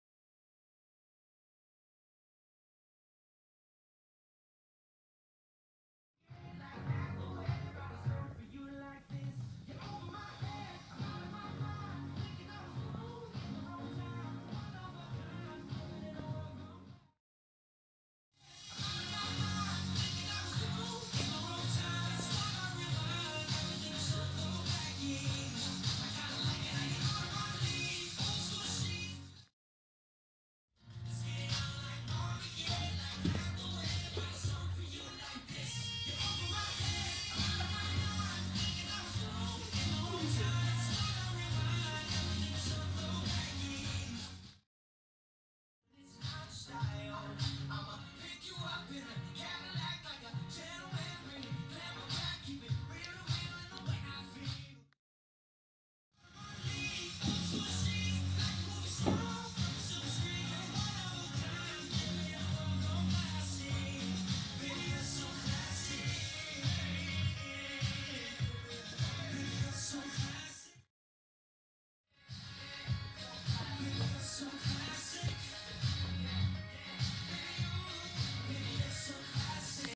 Danssteg
6 olika danssteg:
V-steg
A-steg
Hopp (chassé)
Snurr (pivot turn)
Spark (kick ball change)
Sidsteg (grape vine)